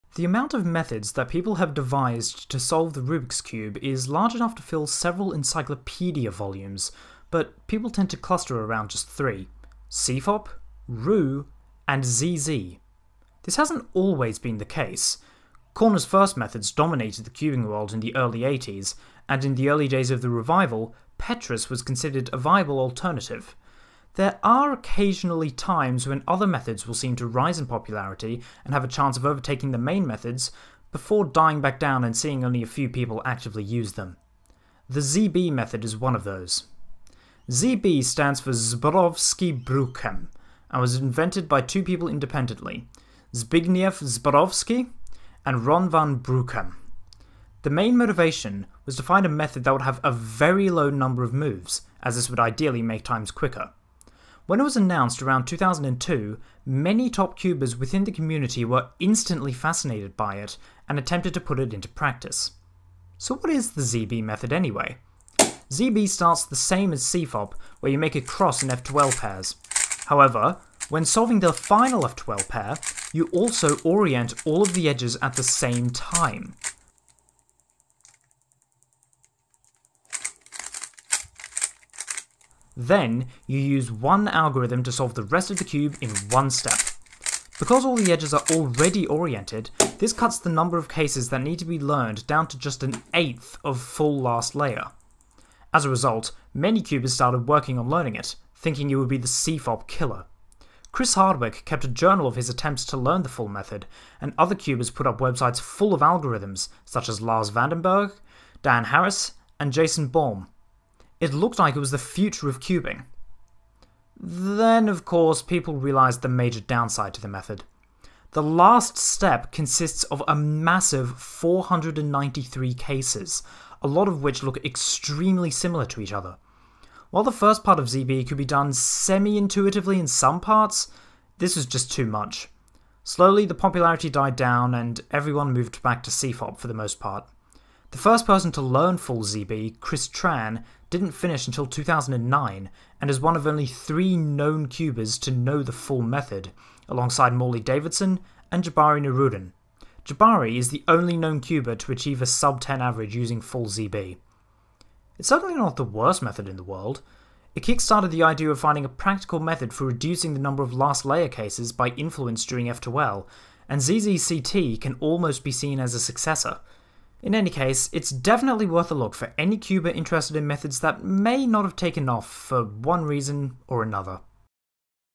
A History of the ZB Method
Back in the early days of the revival, the ZB method seemed like the future. A 1 look last layer every time was a dream for so many people, and this seemed like a practical way to do it. So why did it fail?